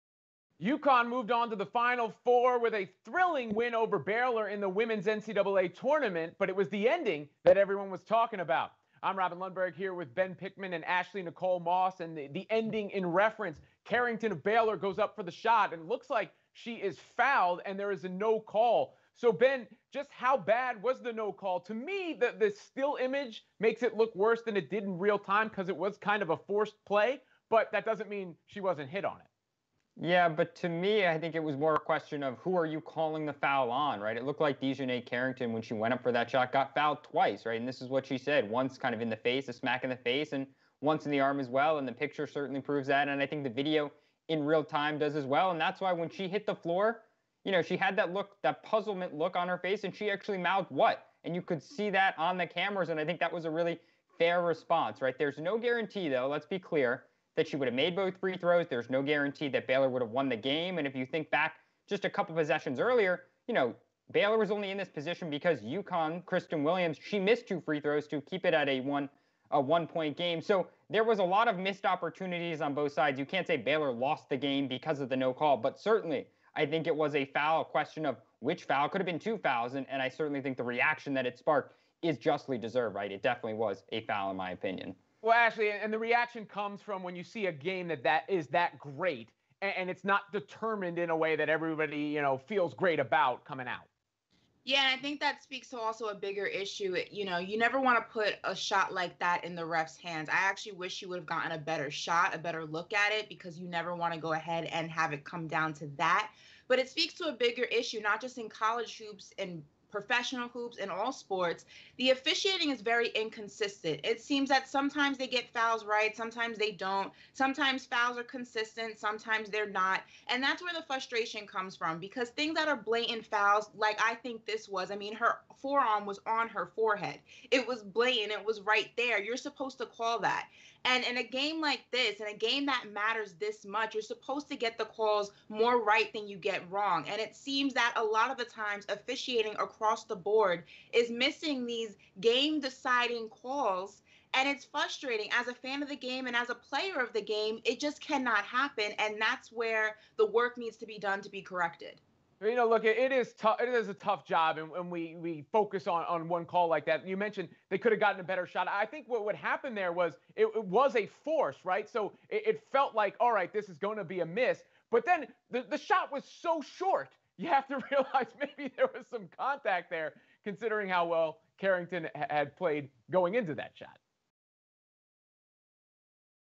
Just How Bad Was The Missed Call In UCONN Vs. Baylor?
UConn defeated Baylor in a thriller to move on to the Final Four of the women's NCAA Tournament, but the win did not come without controversy. A non-call of what appeared to be a foul committed against Baylor's DiJonai Carrington had many questioning the ending of the game. Just how bad was the missed call?